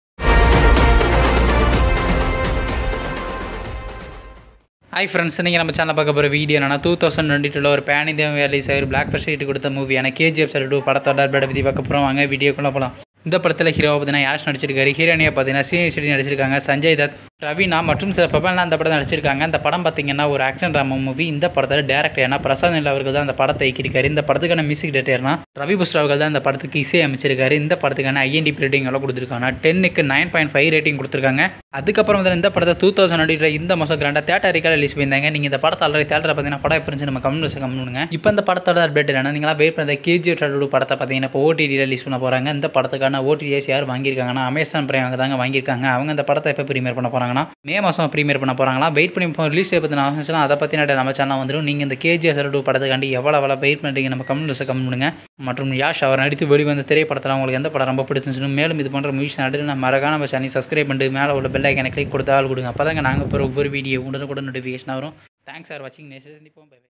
Kgf chapter 2 movie ott release new update tamil | Yash | srinidhi Shetty | Sanjay dutt | prashanth
Kgf chapter 2 movie new update.